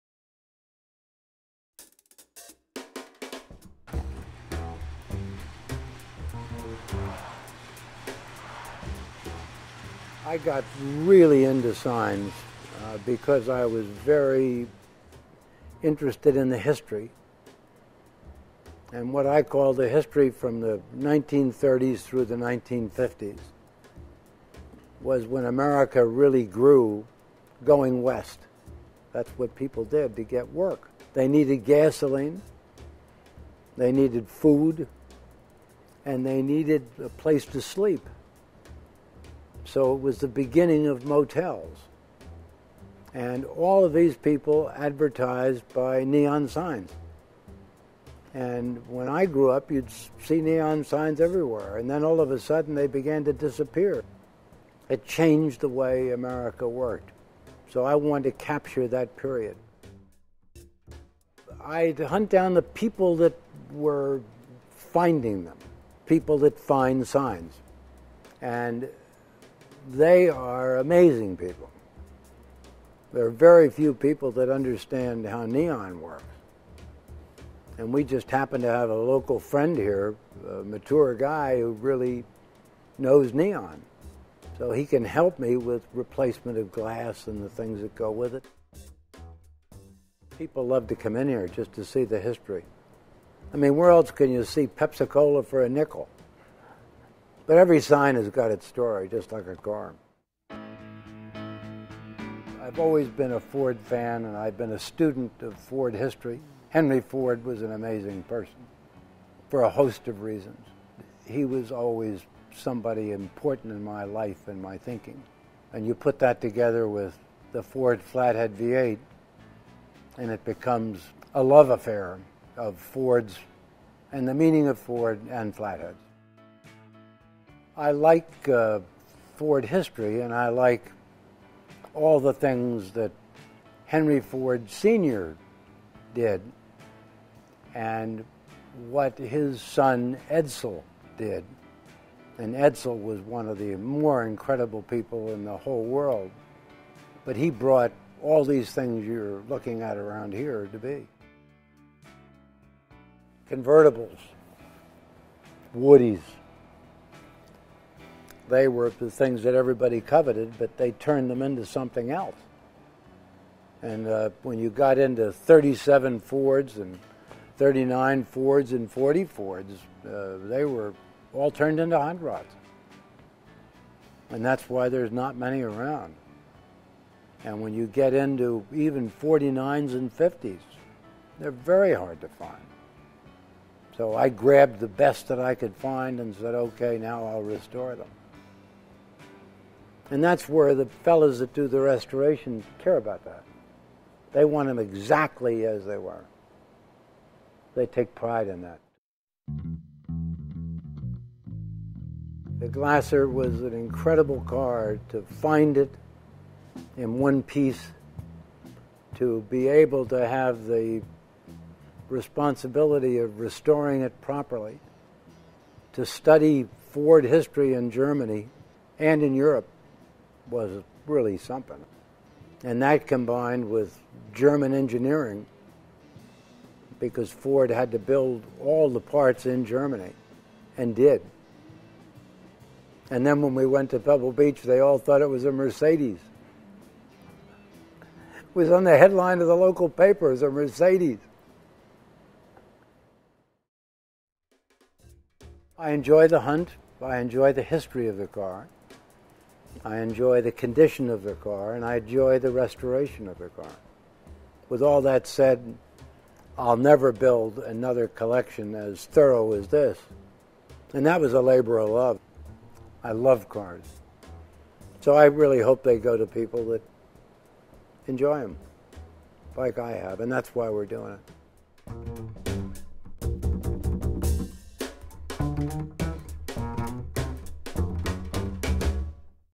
The Dingman Collection 2012
Michael Dingman talks about his remarkable collection, which will be offered at auction June 9-10, 2012 in Hampton, New Hampshire.